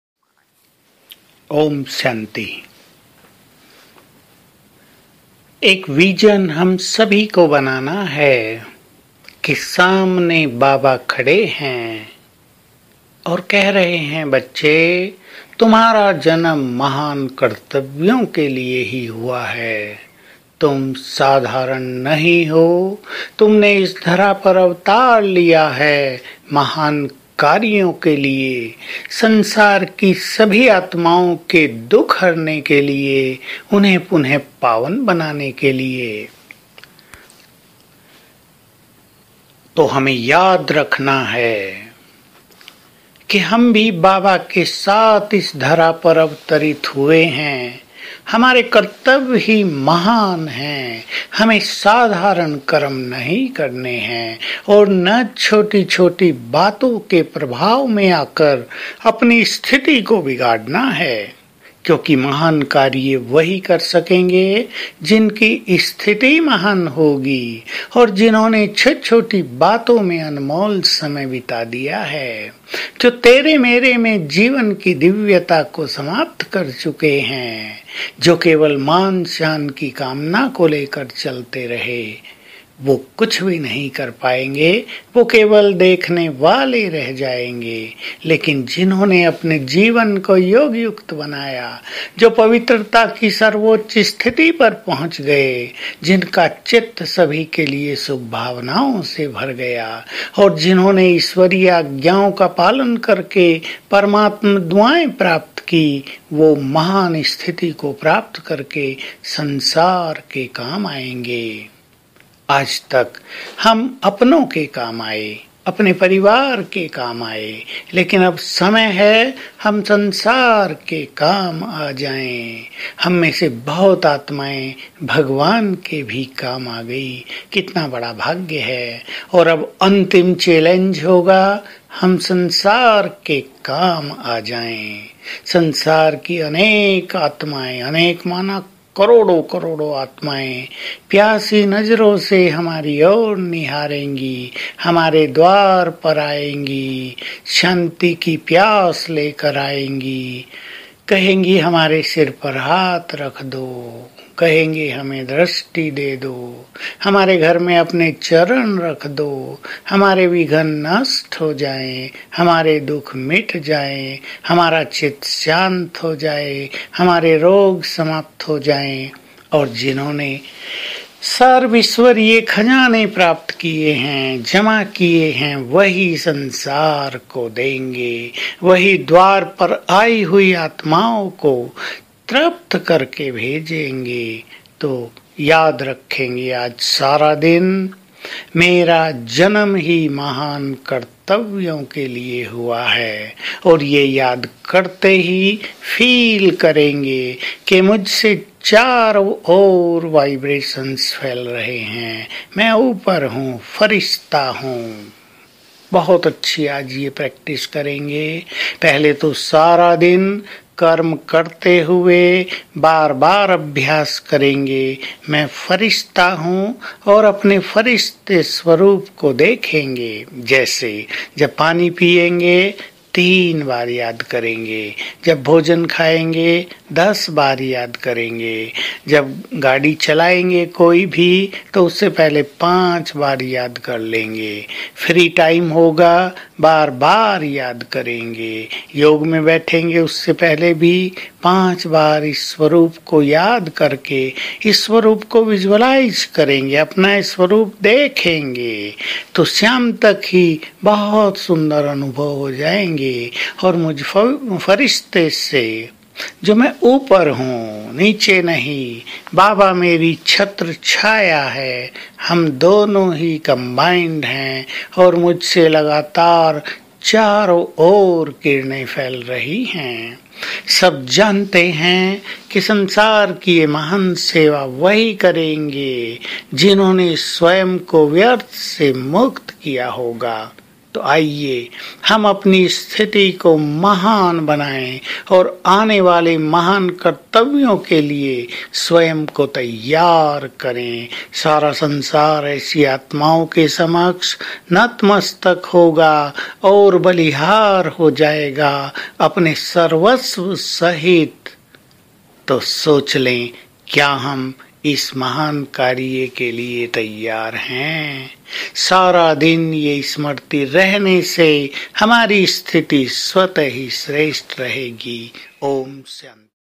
ग्रहस्थ में रहते महान कर्तव्य करने वाली स्थिति बनाने की बेहद सहजविधि और अभ्यास... माननीयBKसूरज भाईजी
ये वीडियो है--पूरे विश्व की मनुष्य आत्माओं, प्रक्रति के पाँचो तत्तो को अलौकिक बनाने की बेहद ईश्वरीय सेवार्थ निमित्त है।